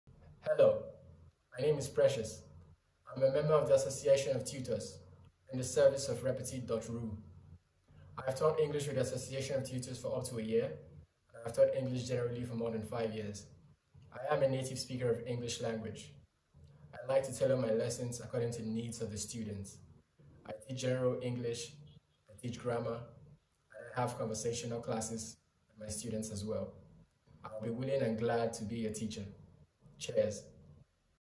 Обенеме Пресиоус Овей - репетитор по английскому языку - видеопрезентация для Repetit.ru
Видеопрезентация репетитора по английскому языку Обенеме Пресиоуса Овея для REPETIT.RU.

Обенеме Пресиоус Овей окончил Курский государственный медицинский университет.